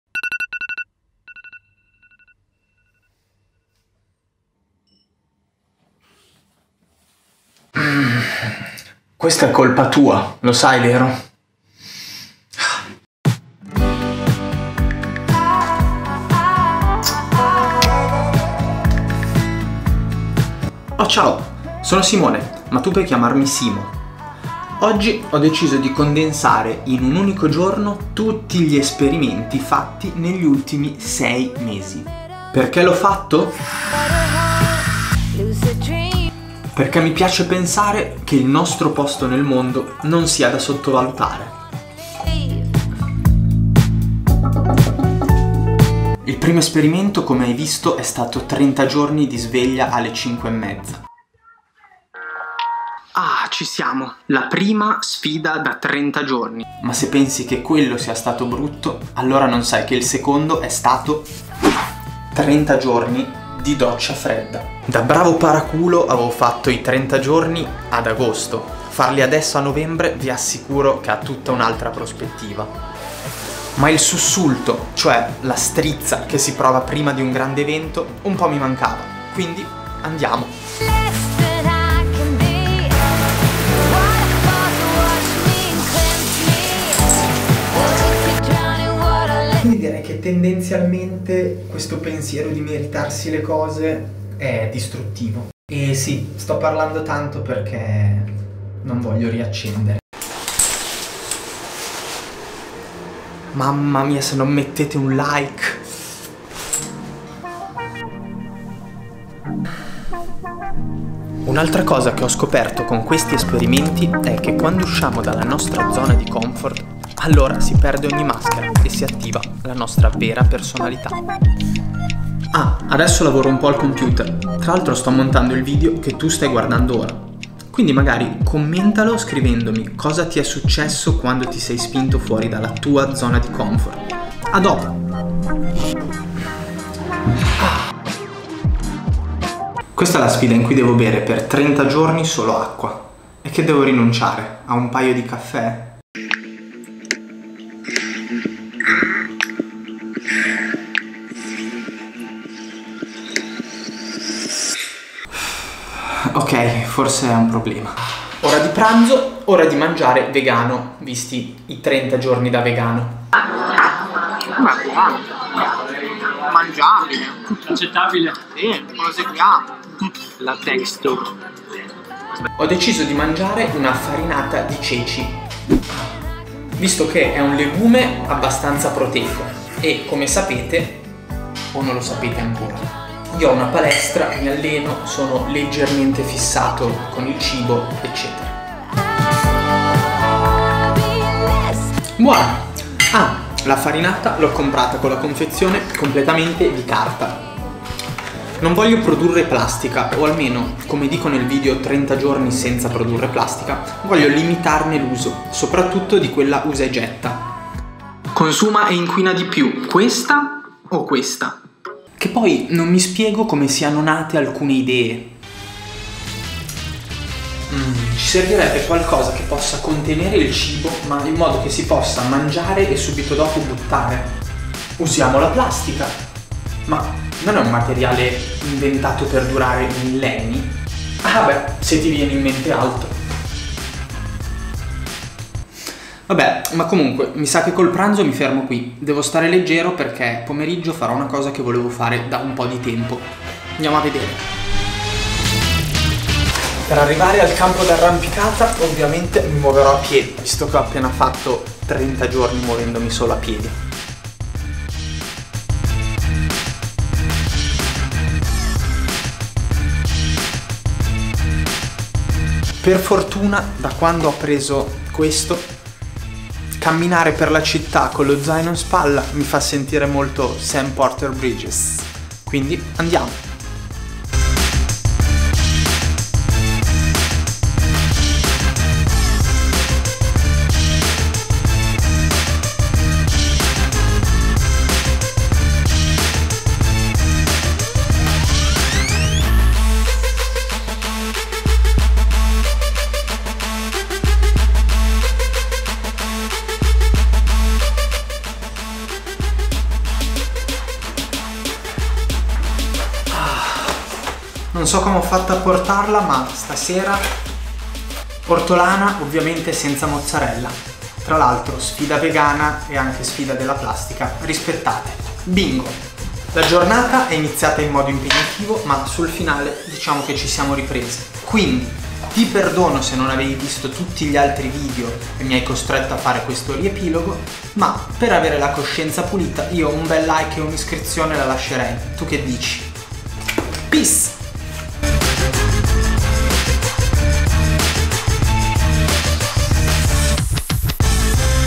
6 MESI delle mie challenge IN UN GIORNO! (Matt D’Avella chi??)
In un singolo giorno condenserò tutti gli esperimenti da 30giorni che ho fatto negli ultimi 6 mesi.

🎥 𝘾𝙤𝙣 𝙘𝙤𝙨𝙖 𝙜𝙞𝙧𝙤 𝙞 𝙢𝙞𝙚𝙞 𝙫𝙞𝙙𝙚𝙤 ➔